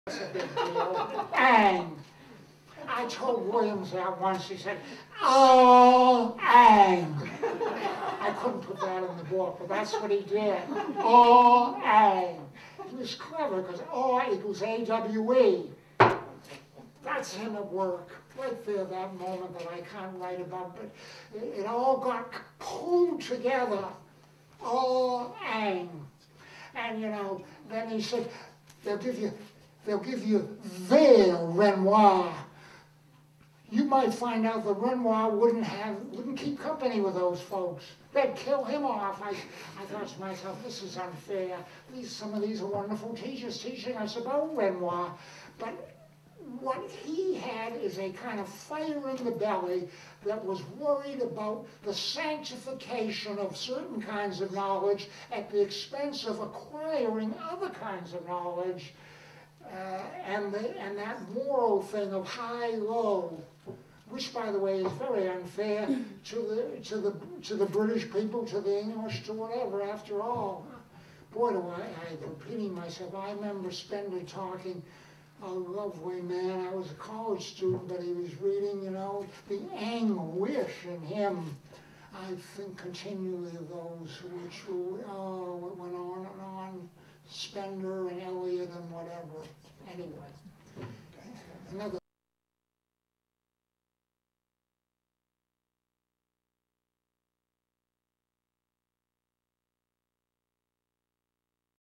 William Carlos Williams Symposium: Part 3 of Talk by Dr. Robert Coles
ON September 20–21, 2008, the town of Rutherford, NJ held a birthday symposium to commemorate their hometown poet’s 125th birthday, which included a wide variety of readings, lectures, and panel discussions by both scholars and members of the Williams family. Dr. Robert Coles gave an informal presentation on “What I Didn’t Put in My Books: A Personal Remembrance of WCW,” - video shared courtesy of the Williams Carlos Williams Symposium